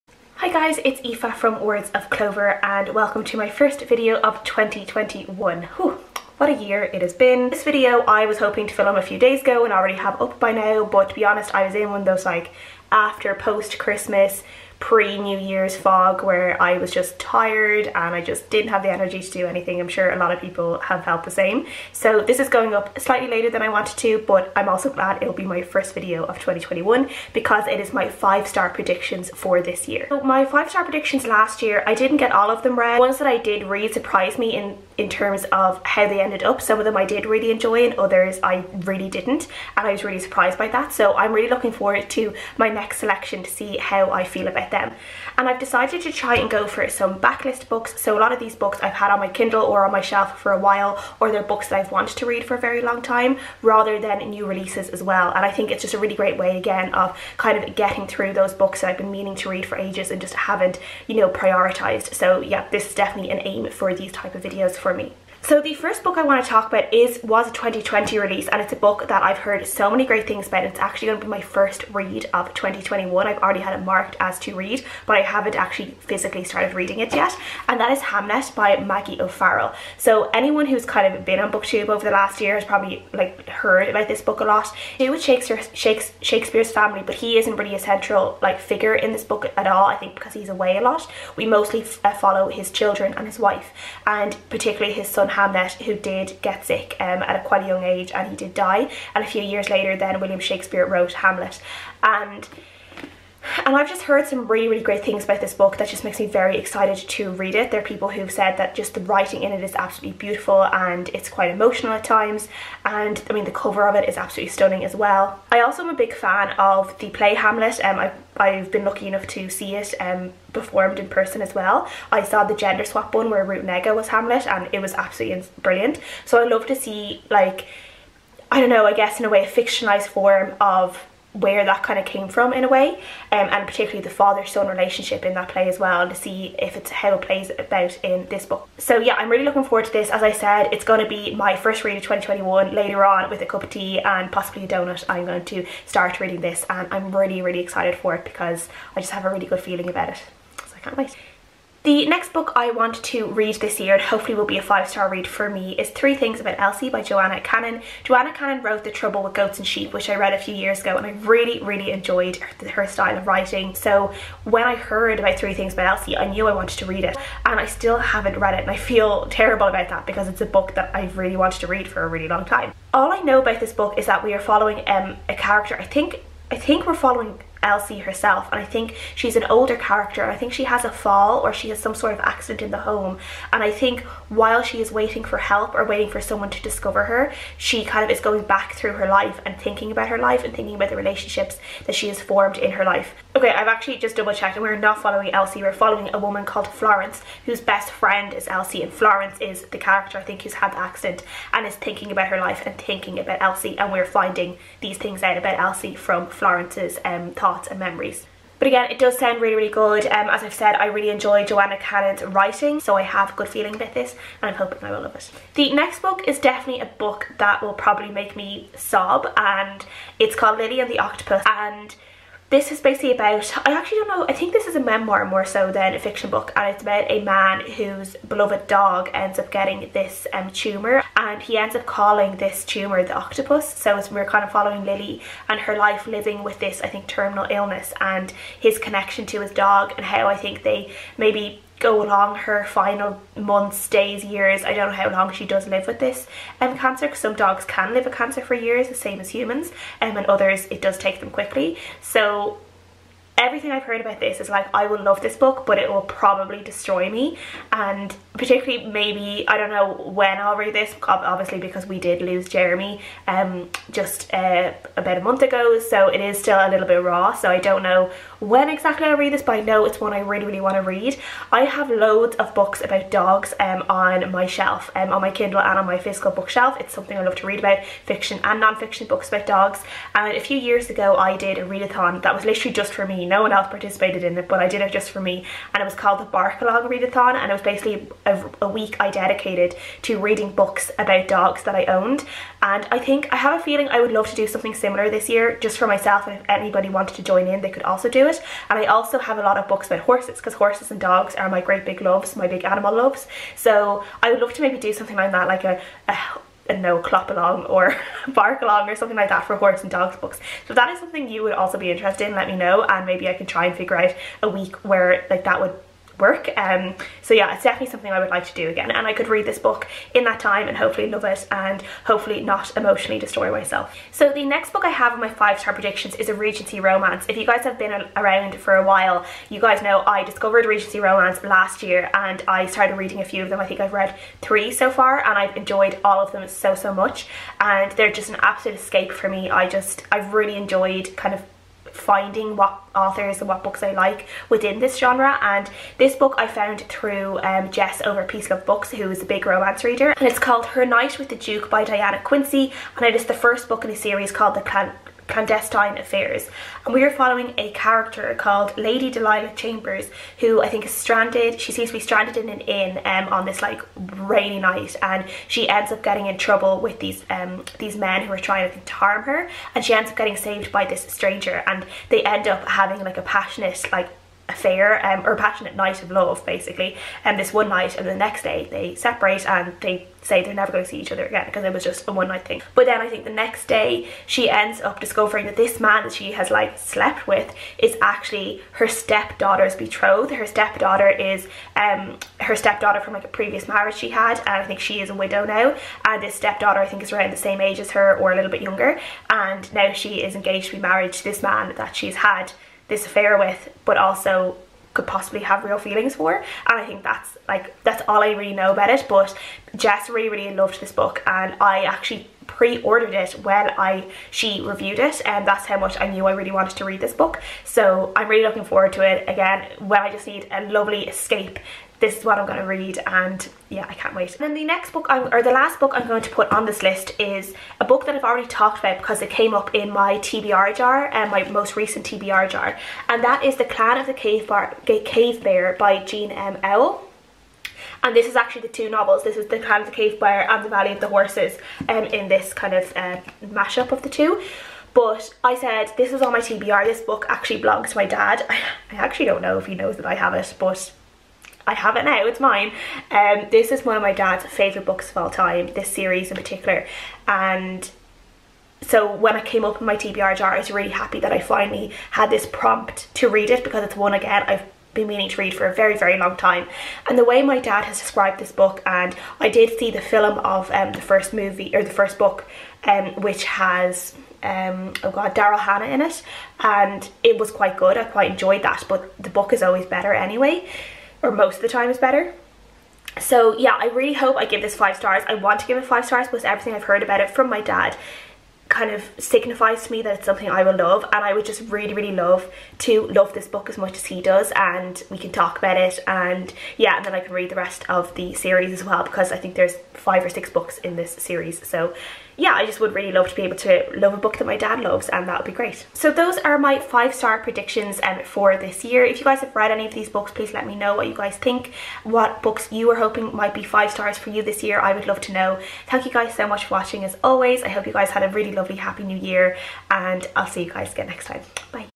5 Star Predictions || January 2021
I've got a good feeling about these books!!

Books:
Hamnet by Maggie O'Farrell
Three Things About Elsie by Joanna Cannon
Lily and the Octopus by Steven Rowley
Her Night With the Duke by Diana Quincy
The Clan of the Cave Bear by Jean M Auel

Amazon Wishlist